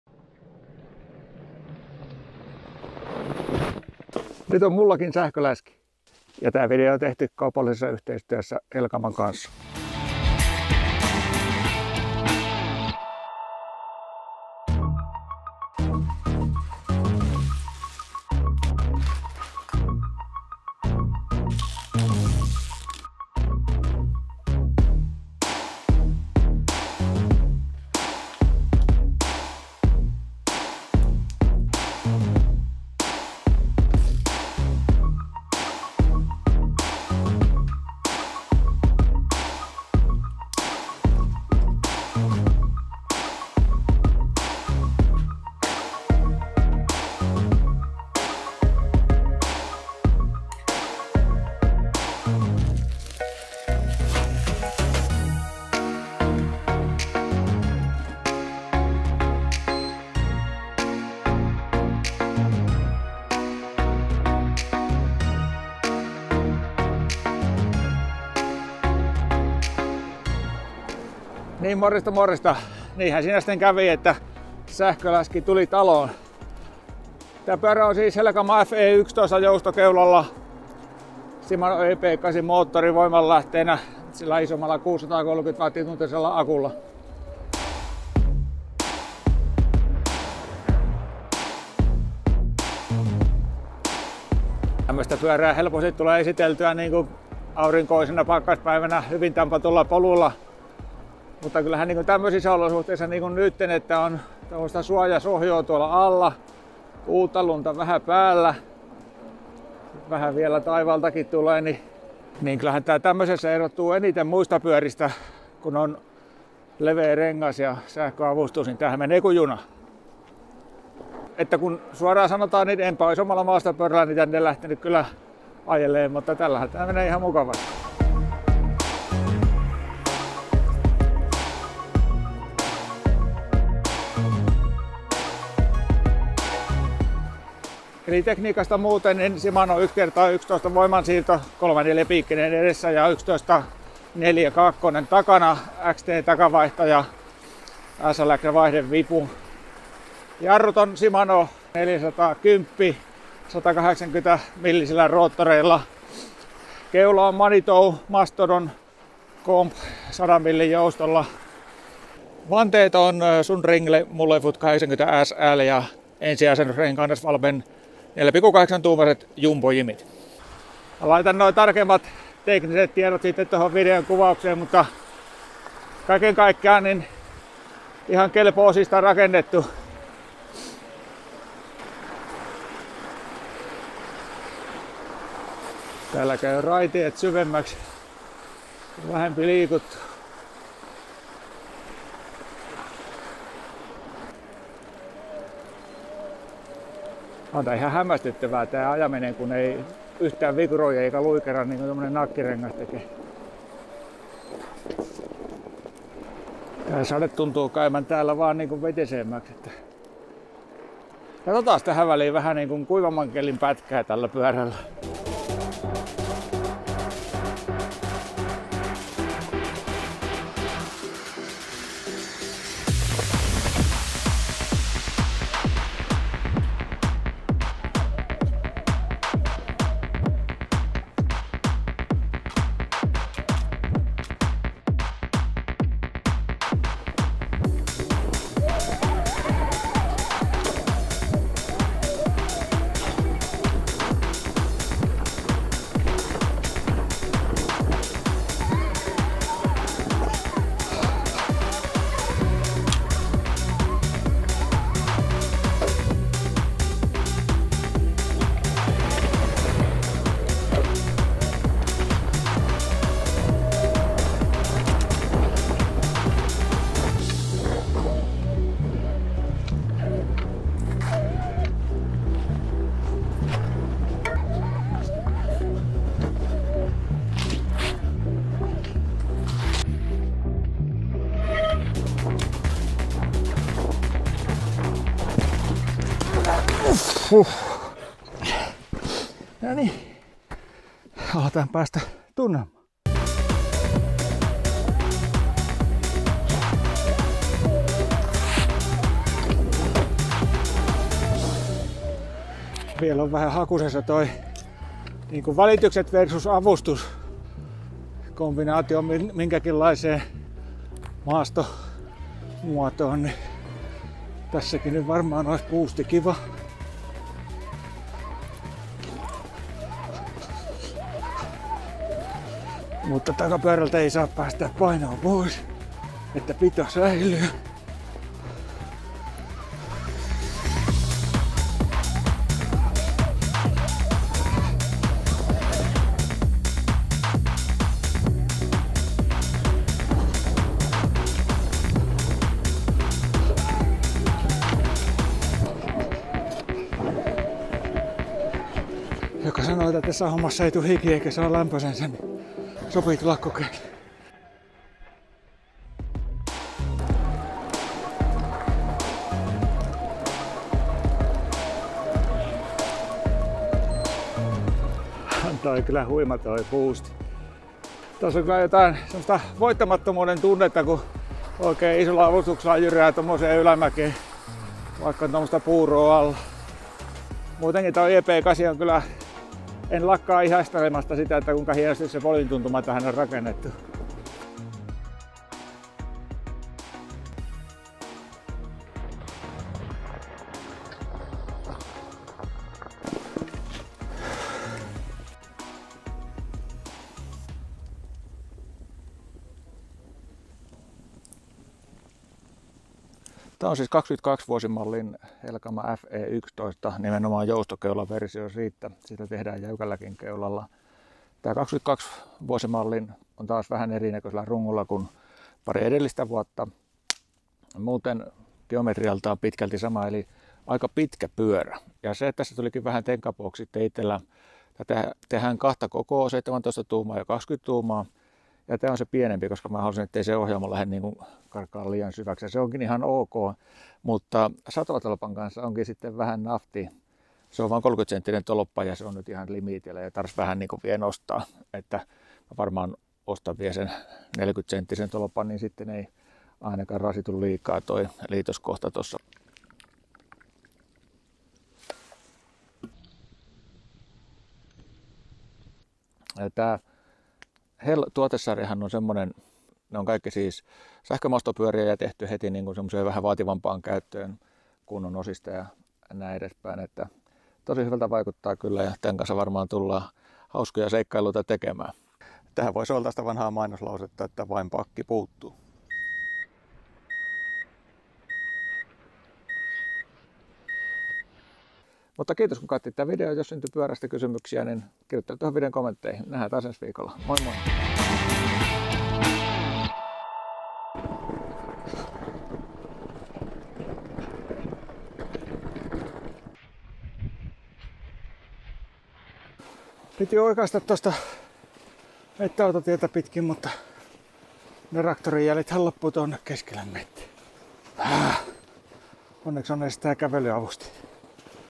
Koeajossa sähköläski Helkama FE11 | Shimano EP8
Esittelen uuden sähköavusteisen läskipyöräni, joka on joustokeulalla varustettu Helkama HEL FE11 !  Video on toteutettu kaupallisessa yhteistyössä Helkama Velox Oy:n kanssa.

Pyörän tekniset tiedot:
Runko Alumiini
Rungon koko 17", 20"
Etuhaarukka Manitou Mastodon Comp 100mm
Vaihteisto Ulkovaihde Shimano RD-M8000 GS XT 11-V Shadow+
Vaihdevipu SLX SL-M7000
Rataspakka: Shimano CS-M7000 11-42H
Kampisarja Samox e-Fat 34H CL7, ketjusuojalla
Ketjut Sunrace
Etujarrut Shimano MT-410 hydraulinen levyjarru
Takajarrut Shimano MT-410 hydraulinen levyjarru
Jarrulevyt: SM-RT66 180mm 6-pultti
Jarrukahva Shimano
Istuin Selle Royal Essenza Athletic
Istuinkannatin Kiinteä 30,9x300mm musta
Ohjaustanko Alumiini 35/780
Ohjainkannatin Kiinteä 35mm
Renkaat: Schwalbe Jumbo Jim ETRTO 120-559
Vanteet: Sunrinle Mulefut 80SL 559 32H Tubeless ready
Etunapa: KT-M9CF 32r 15x150 mm läpiakseli
Takanapa: KT-M9ZR 8-11v 32r 12x197 mm läpiakseli
Moottori Shimano DU-EP8
Moottorin teho 250W-36V-85Nm
Akku malli Shimano BT-E8036
Akun energiamäärä  630
Akun sijainti Integroitu
Akun latausaika 6
Laturi Shimano EC-E6000
Näyttö Shimano SC-EM800

Haluatko tukea kanavaa ja saada siltä lisäominaisuuksia? Tilaajana voit liittyä kanavan jäseneksi